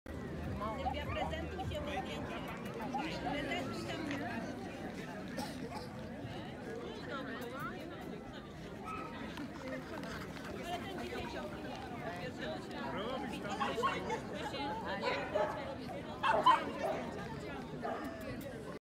28 Club Show Wrocław 19.10.2019 - Magnetic Forse
28 Club Show of Spitz and Primitive Breeds Wrocław
28 Klubowa Wystawa Szpiców i Psów Pierwotnych Wrocław
sędzia/judge: Maureen Thompson (ENG)